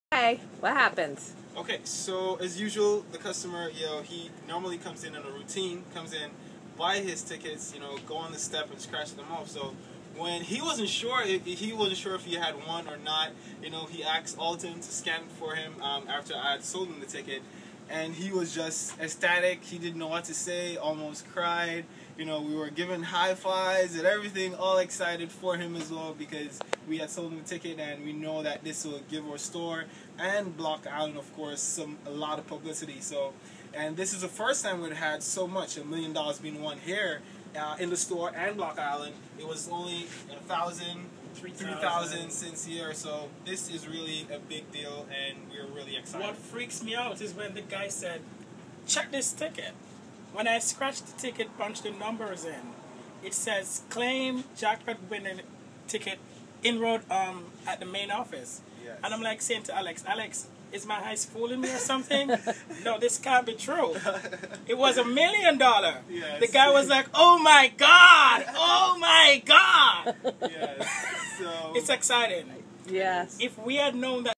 N.J. Man wins $1 million lottery ticket on Block Island
A N.J. man on vacation on Block Island won $1 million when he bought the "$1,000,000 Scratch" instant ticket on Monday, July 14.
Alex McWilliams and Alton Bentley were working at the Block Island Pharmacy when the man bought the ticket and won. Reporting and video by Kari Curtis.